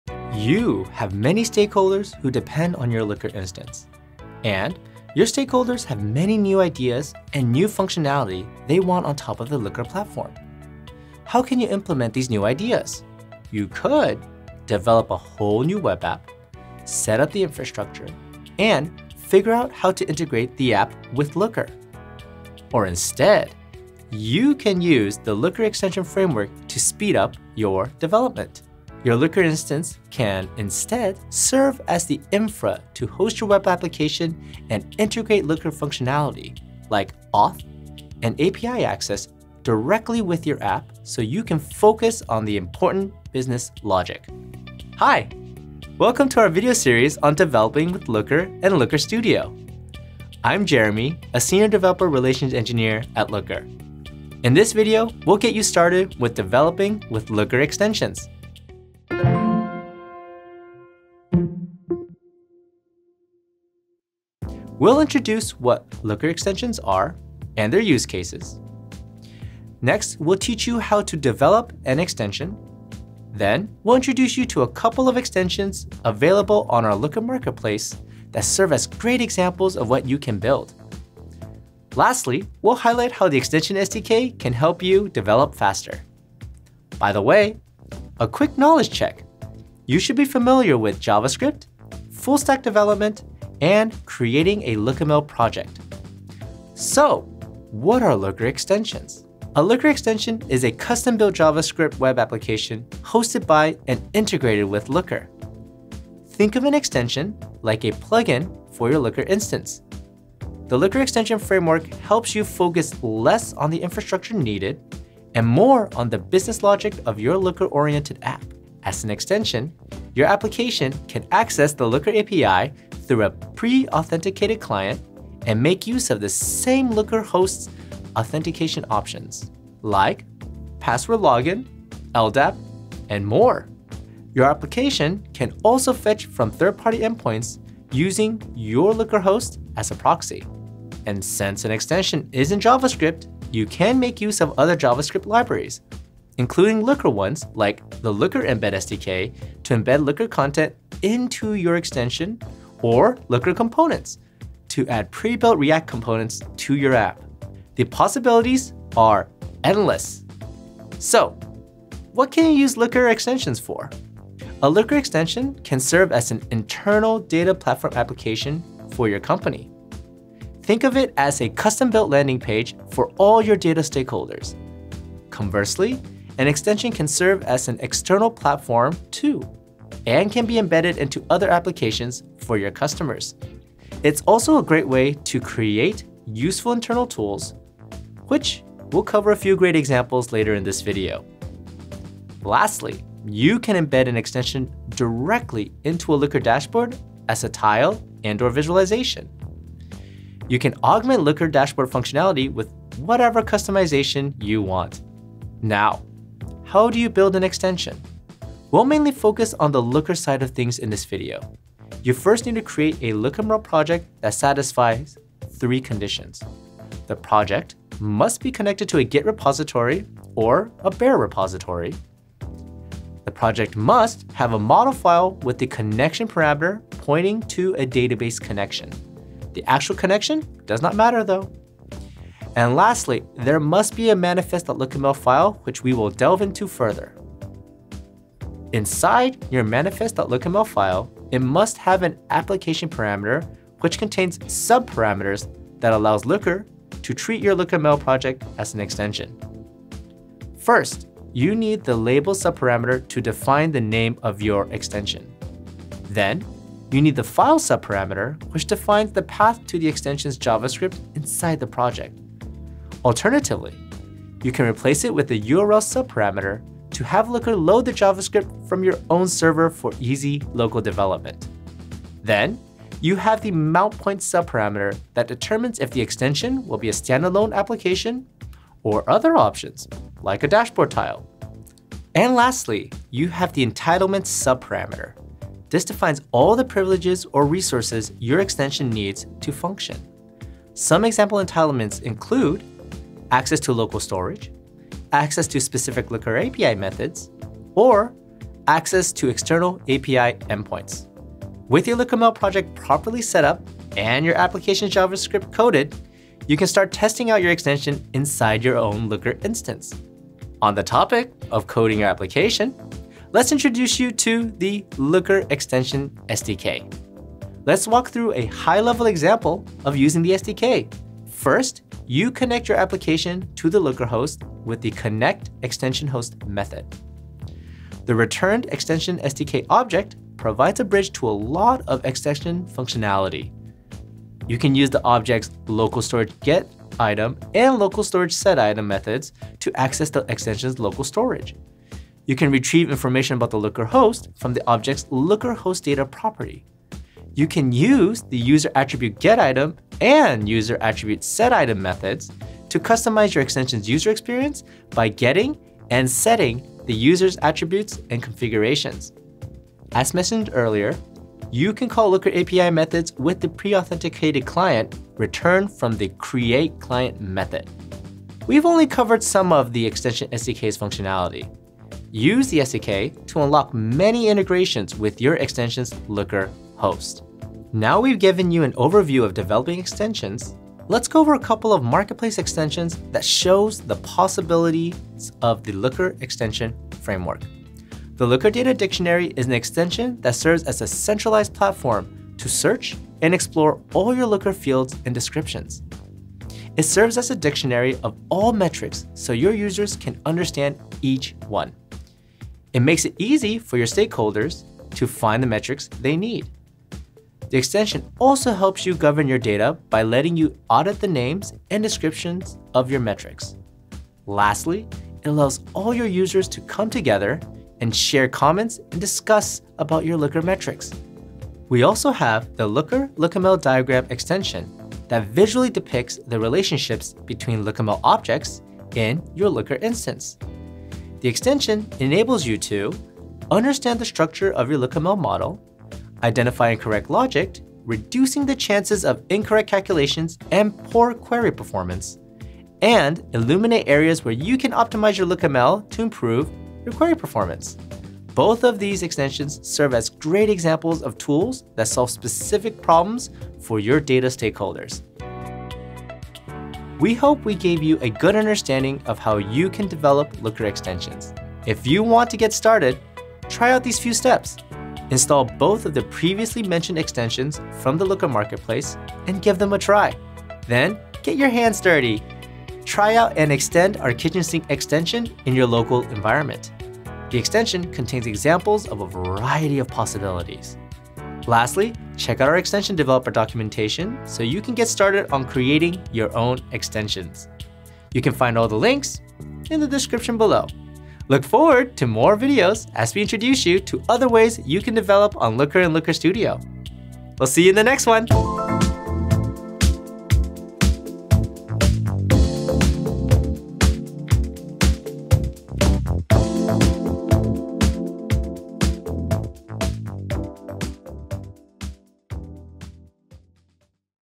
Develop with Looker Extensions
Build custom data web applications quickly on the Looker platform. In this episode of Develop with Looker and Looker Studio we walk through how to add custom functionality to Looker with the Looker extension framework. Watch to learn how you can develop applications integrated with Looker.